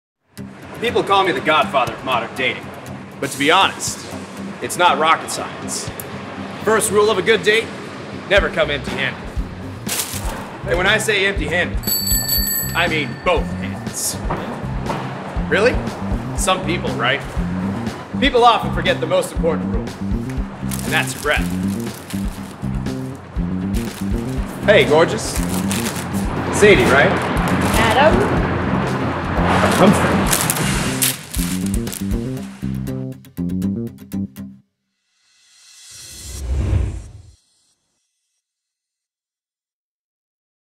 LISTERINE: "Bad Breath" AD
Humphrey has always considered himself a dating master. Here are some of his top tips for success in the romantic world.